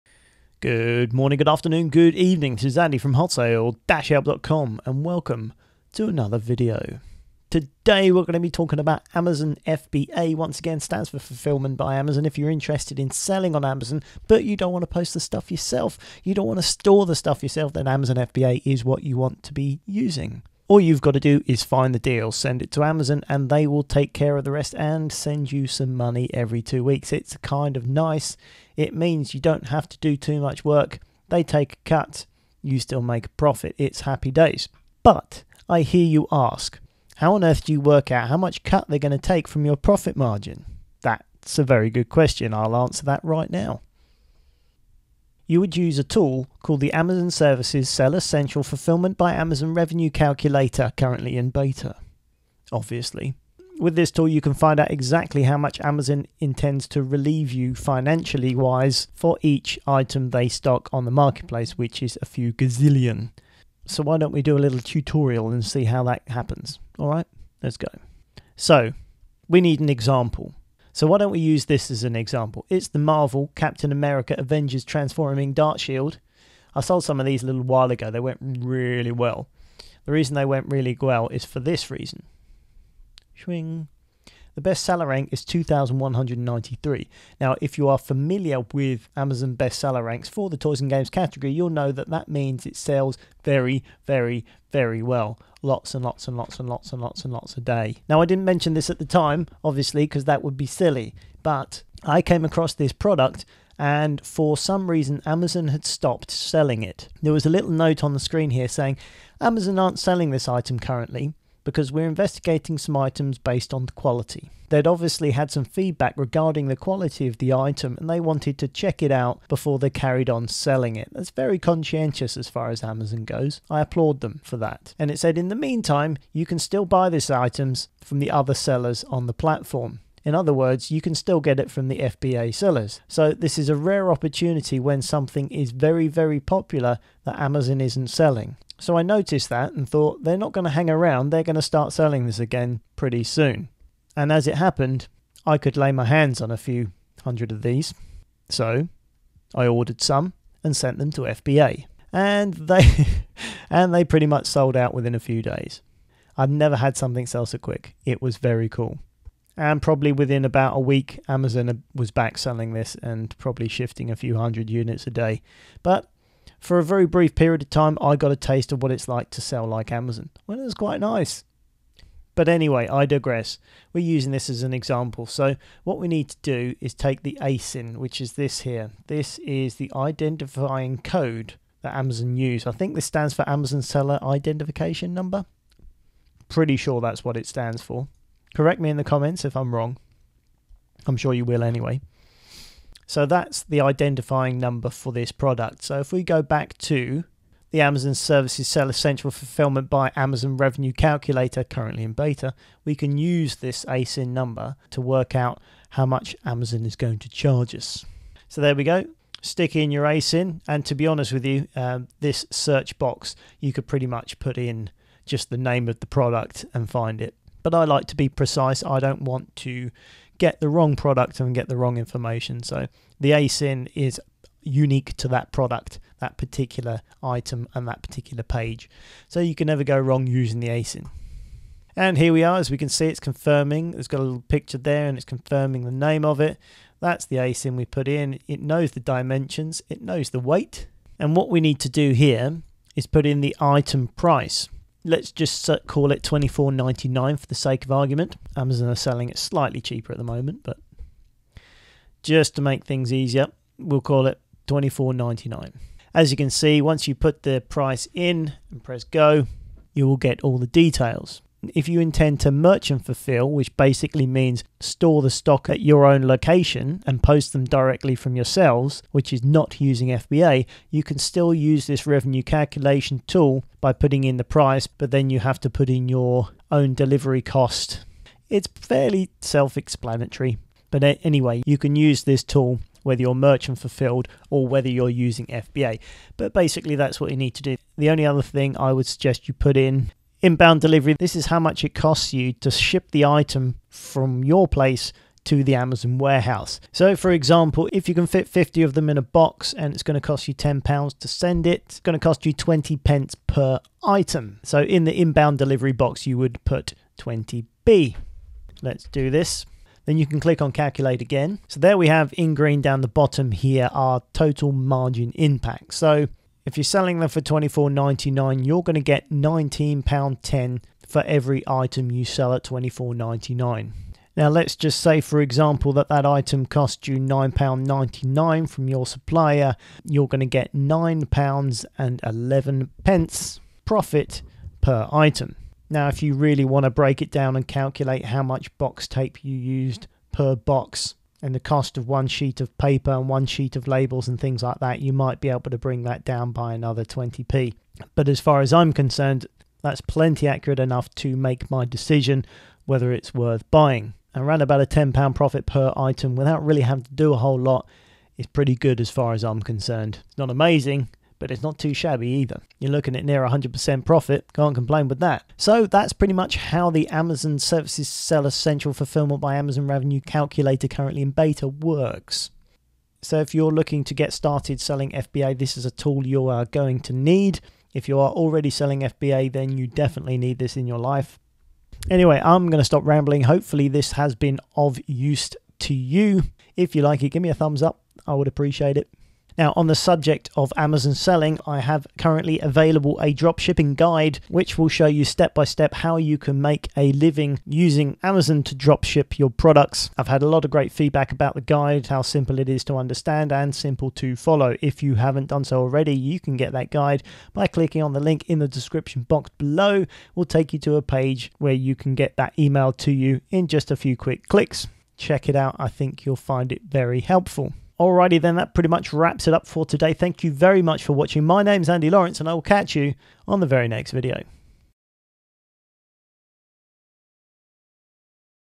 How to Use The Amazon FBA Revenue Calculator Tool
If you're an FBA seller or considering selling using the Amazon FBA platform then this is an essential tool in your armory. This tool will help you work out how much Amazon is going to charge you to fulfill your items which is essential to know when working out how much profit you are going to make on your items. So here is a quick demo and tutorial of the Amazon Revenue Calculator Tool.